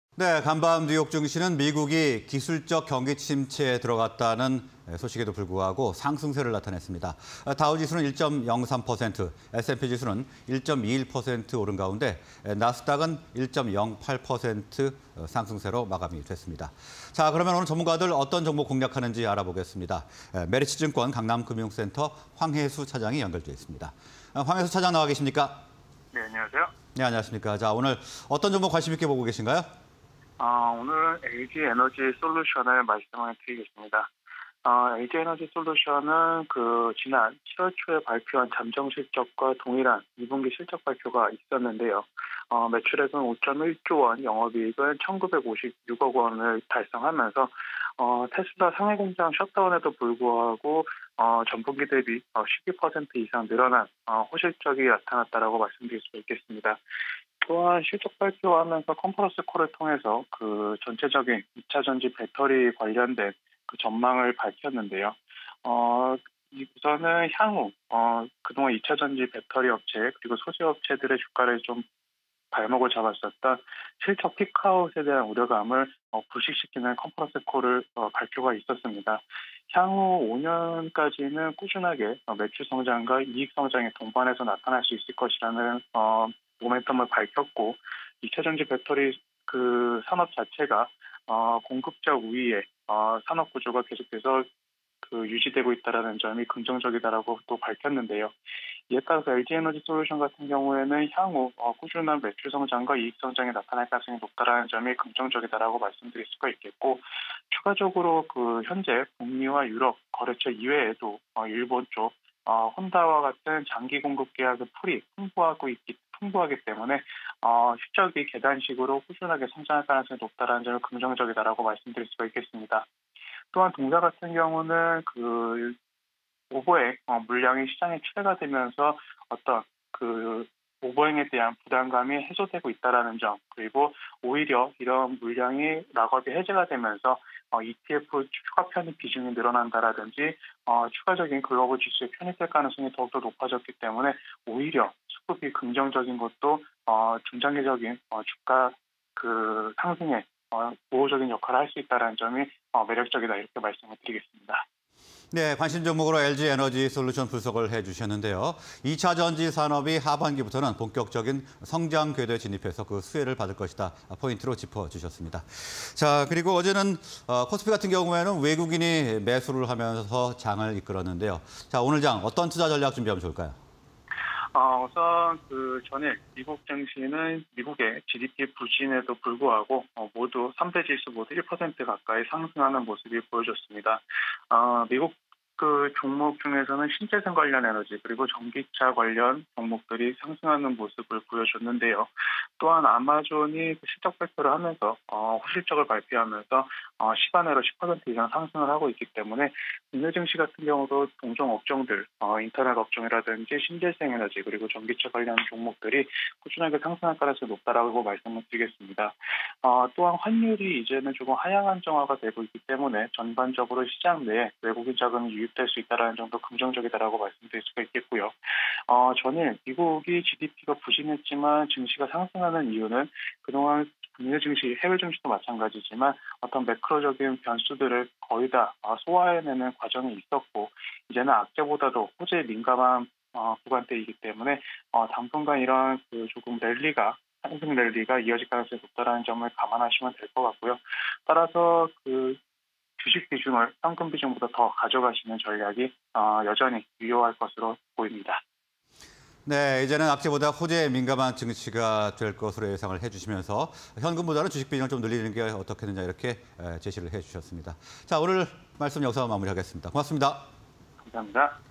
LG에너지솔루션(373220)... 하반기부터 본격적인 성장 궤도 진입_굿머니 공략주 (20220729)
LG에너지솔루션(373220)
- 하반기부터 본격적인 성장 궤도 진입
- 오버행 물량 출회로 불확실성 완화
- 2차전지 수요 견조… 피크아웃 우려 완화